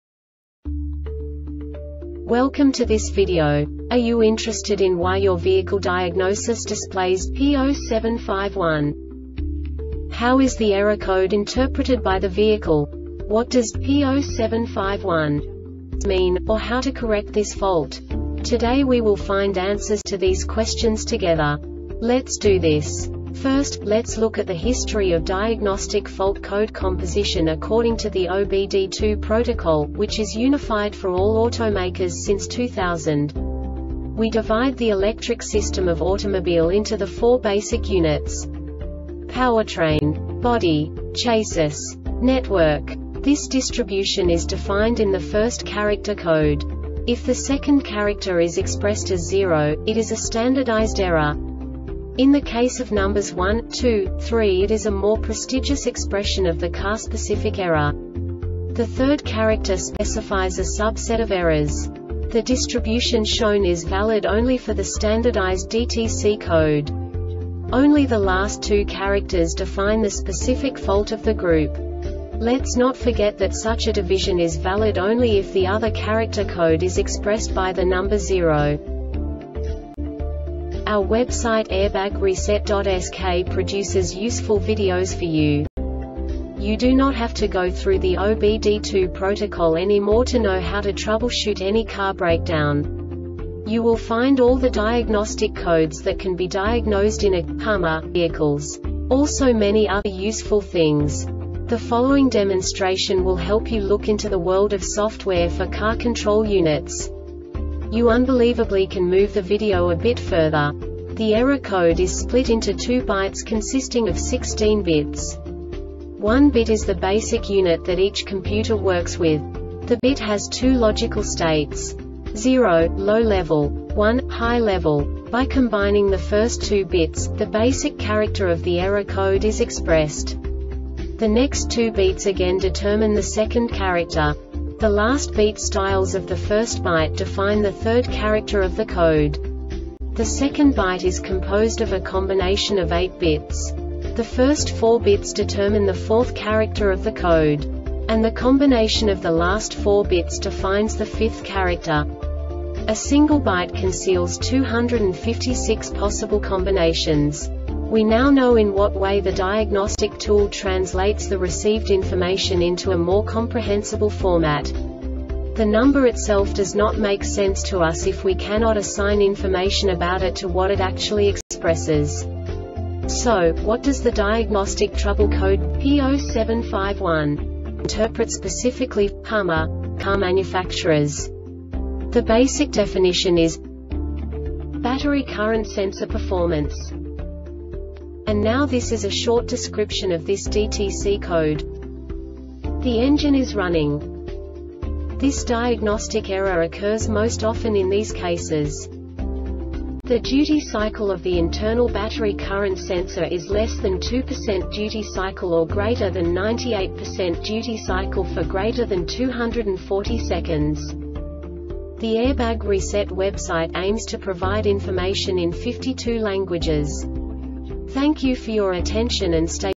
DTC Hummer P0751 Short Explanation
Contents:
0:21 Basic DTC analysis according to OBD2 protocol standard.
1:48 Insight into programming
2:58 A diagnostically understandable description of the Diagnostic Trouble Code
3:05 Basic error definition

Make:
HUMMER
Code:
P0751
Definition:
Battery Current Sensor Performance
Description:
The engine is running.
Cause:
The duty cycle of the internal battery current sensor is less than 2 percent duty cycle or greater than 98 percent duty cycle for greater than 240 seconds.